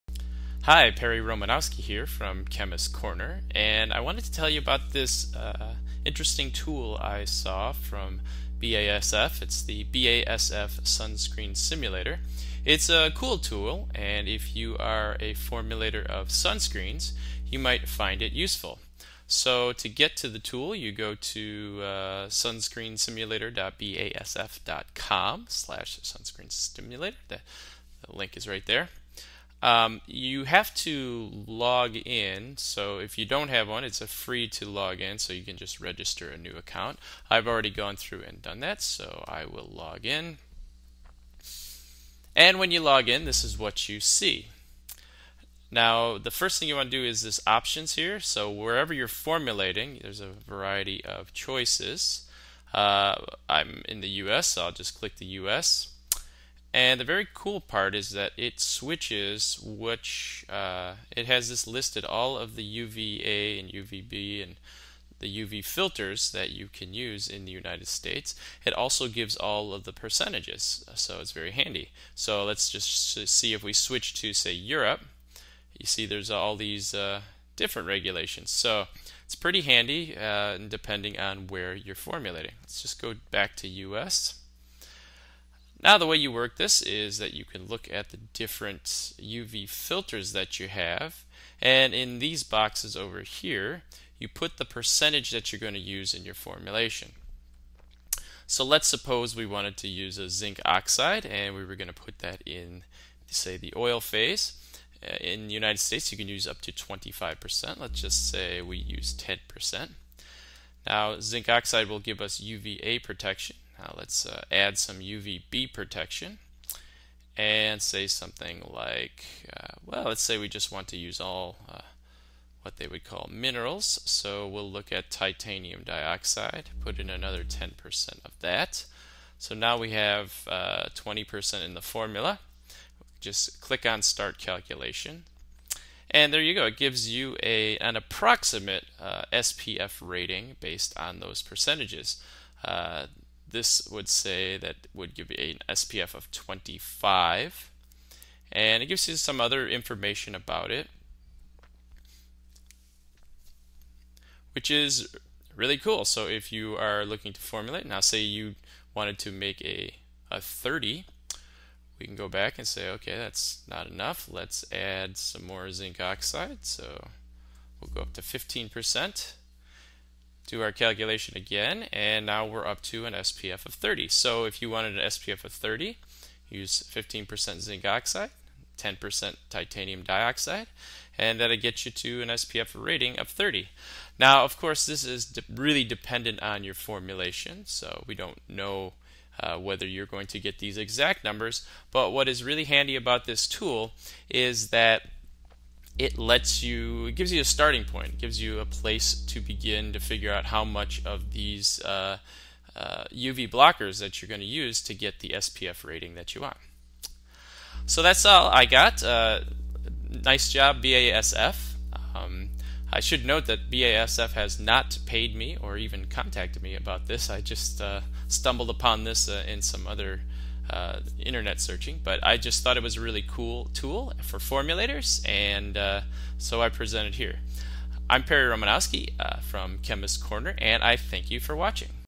Cosmetic Science - Sunscreen Tool
A free online tool that lets you calculate exactly how much active sunscreen ingredients to use to get a certain SPF rating.  A useful guide.